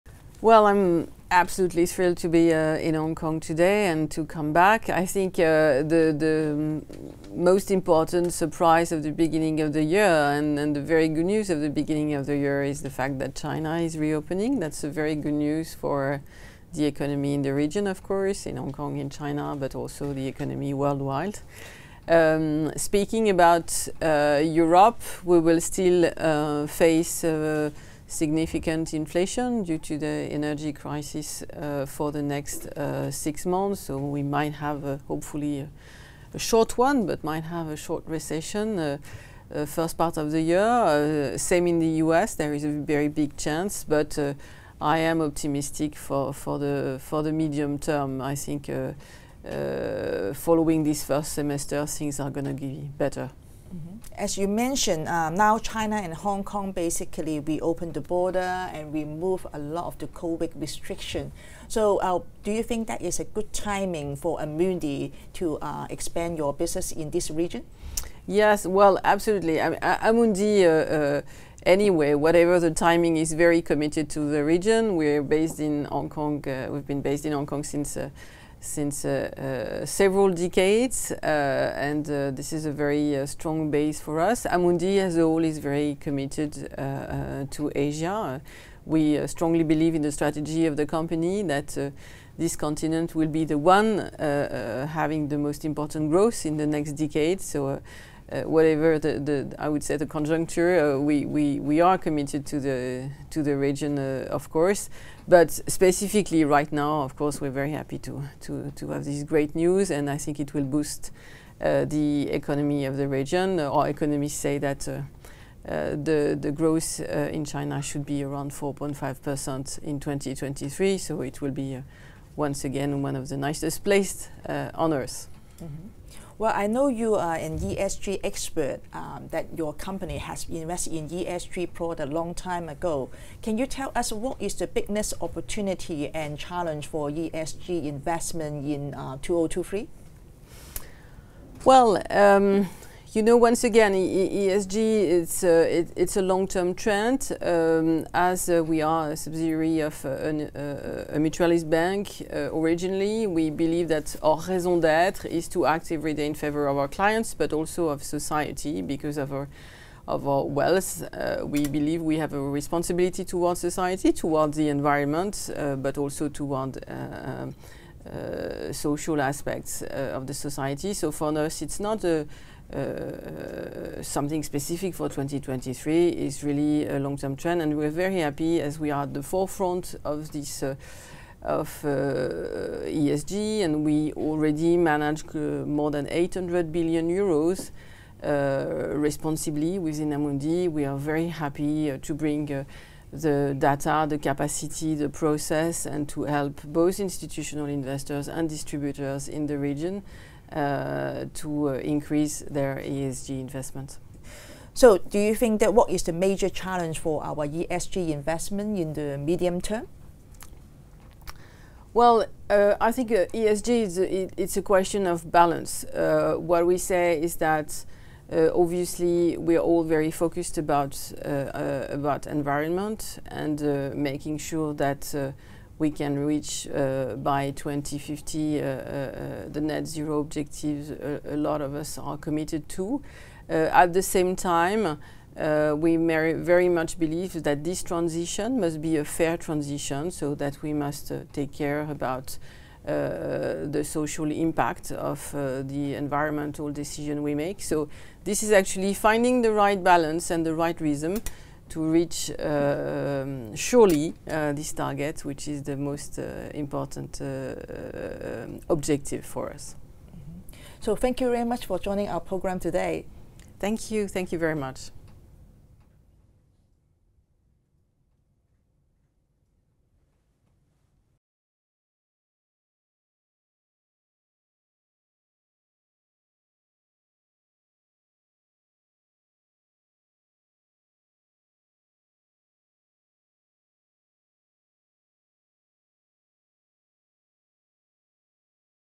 Europe’s largest fund house Amundi upbeat on Asia economic, market outlook after China reopening
Valerie Baudson, CEO of Paris-headquartered Amundi, said China’s reopening is very good news for the economy in the region and worldwide. China’s reopening will spark economic growth to reach 4.5 per cent in 2023, she said. The target will be far higher than the US and Europe, which may experience a short recession and high inflation due to the energy crisis, she said. Amundi is the largest asset management company in Europe, while it has been operating in Hong Kong for more than four decades.